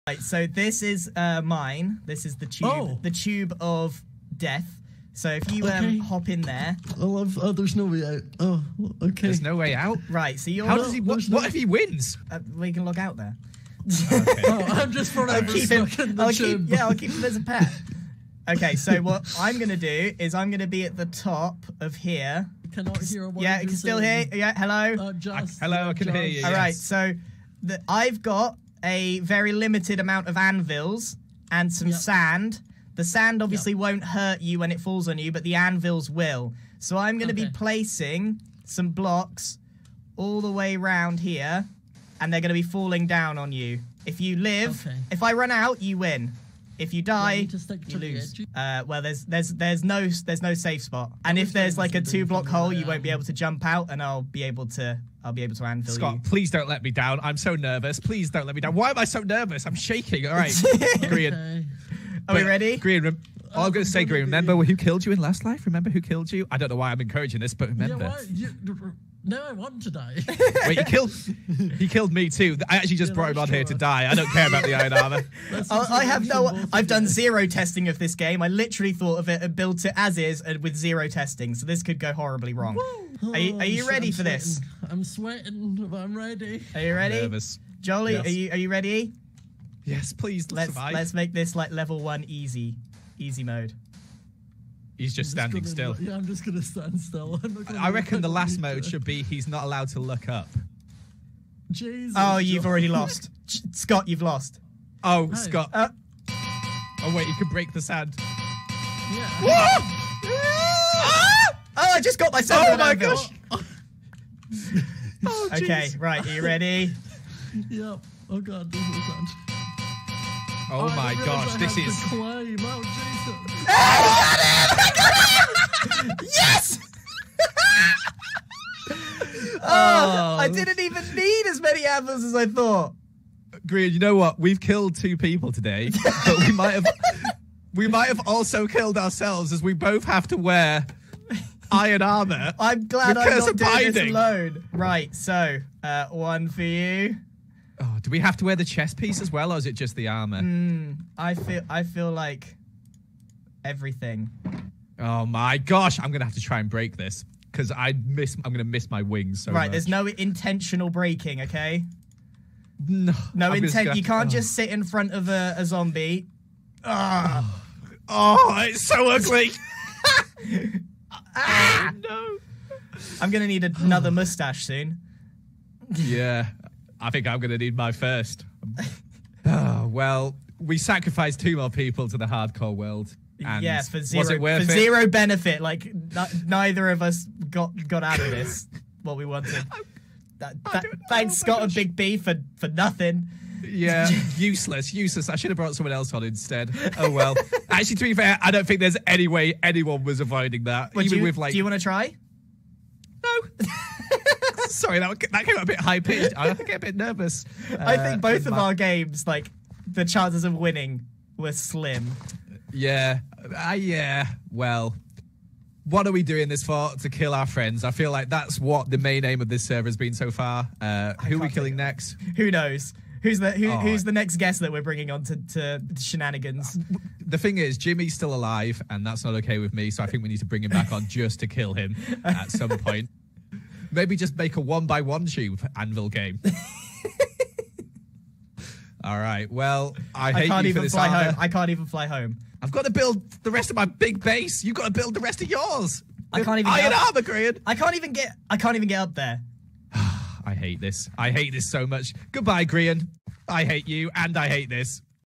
Smajor1995 Challenge for Smallishbeans loosing all his armor for iron permanently! - 100 Hours
Original Source - Grian